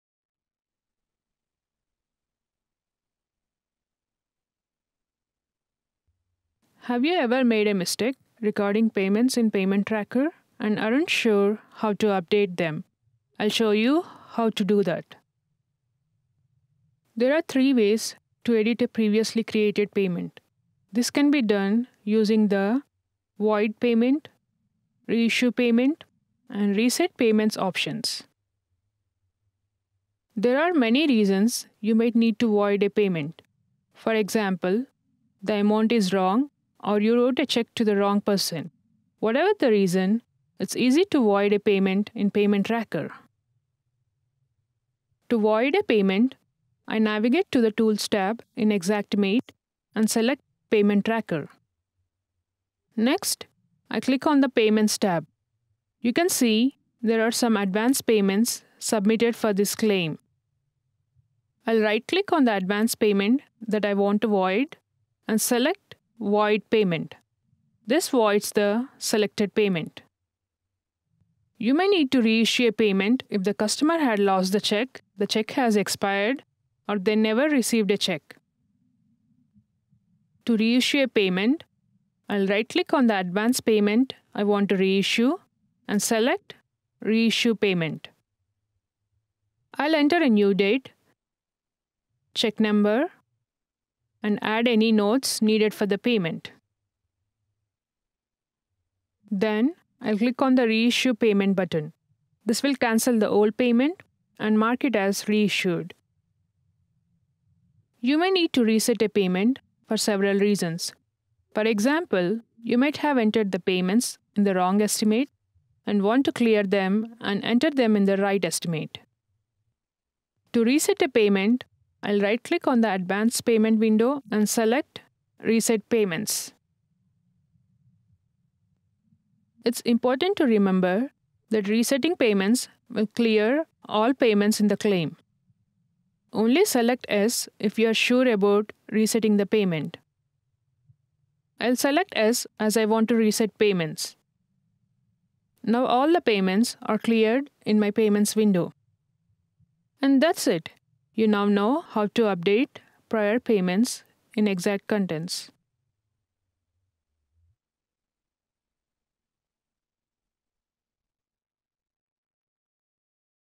Xactimate Xpert Tip: Updating Prior Payments in XactContents
Learn how to update prior payments in XactContents. Xactimate Desktop Version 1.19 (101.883)